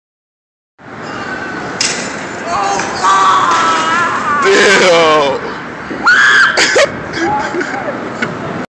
Major bike fail
Lil bro fails at riding bike down slope.